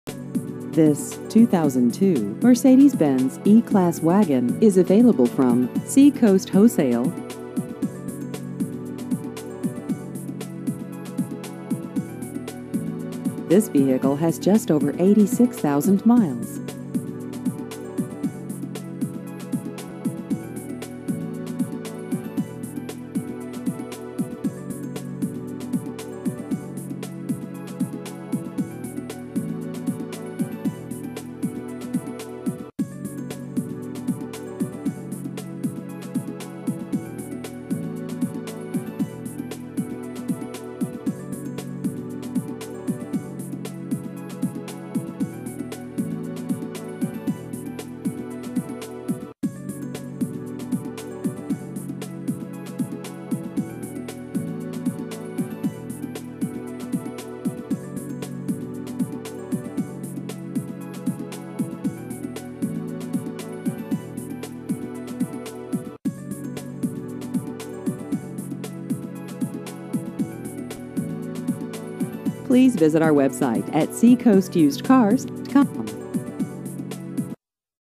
2002 Mercedes-Benz E-Class Wagon Used Cars Hampton Falls NH
This 2002 Mercedes-Benz E-Class Wagon is available from Seacoast Wholesale.